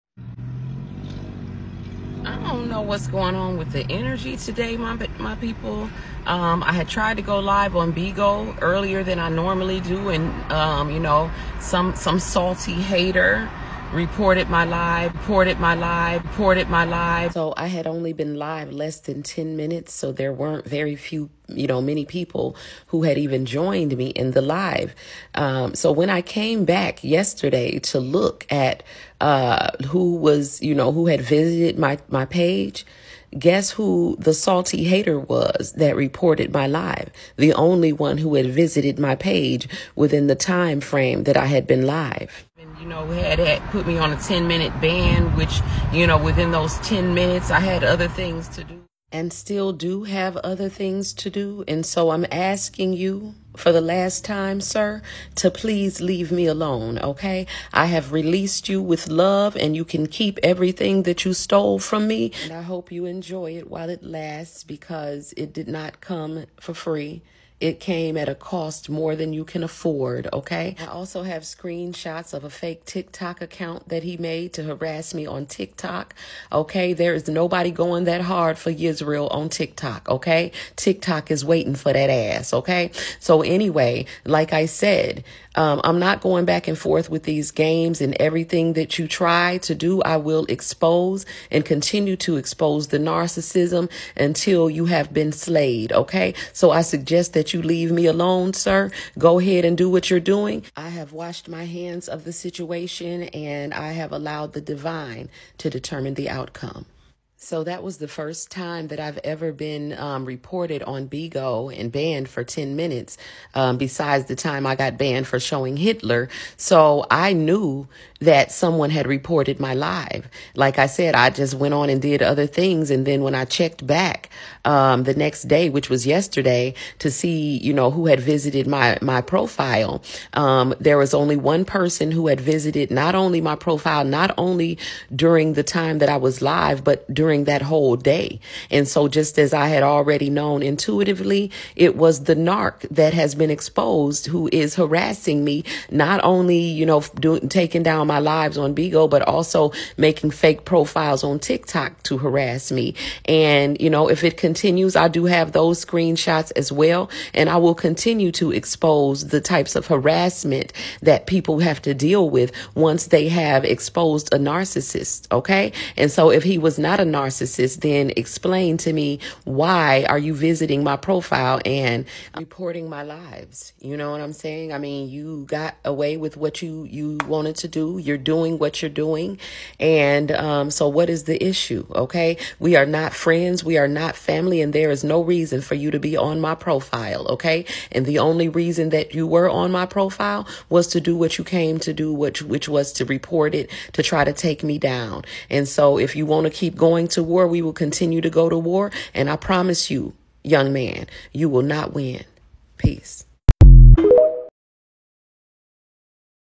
Covert Narcissism After Exposure
The covert narcissist is relentless in their efforts to destroy you. Cut all ties with them and anyone associated with them as they can and will be used against you in this warfare.